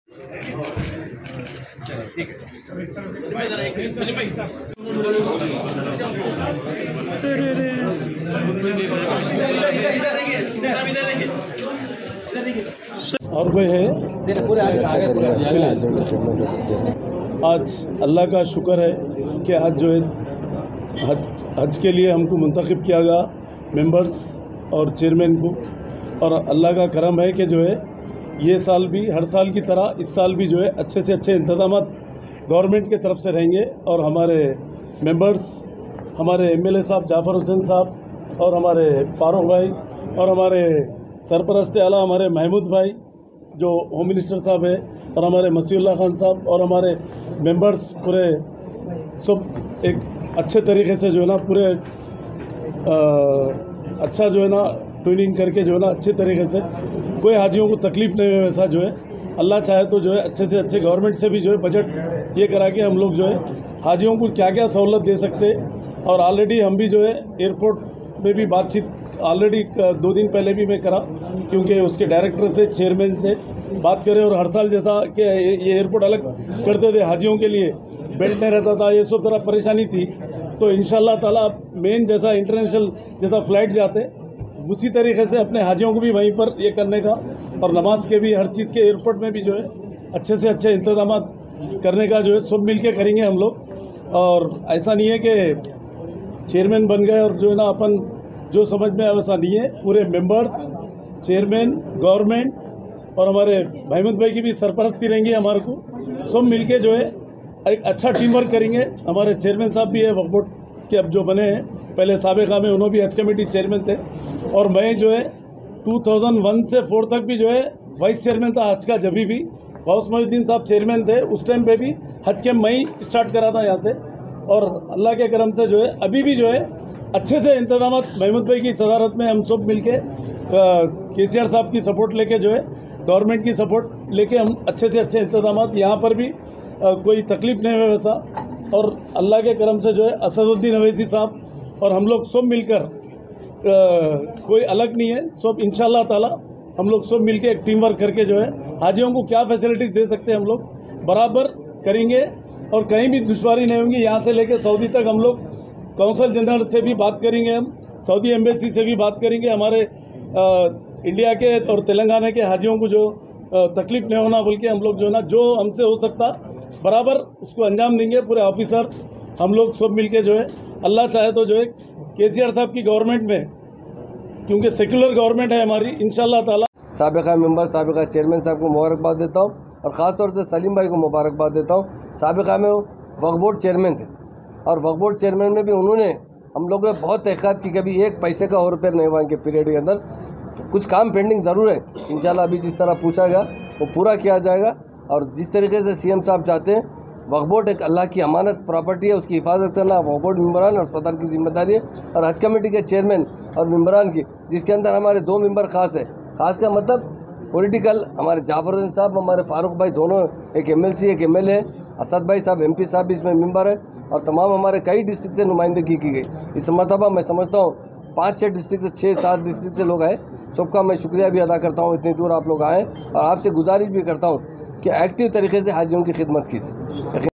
T Express News# Mohd Saleem Takes Charge as Chaiman Of Telangana Haj Committee.
Mohd Saleem Takes Charge as Chaiman Of Telangana Haj Committee.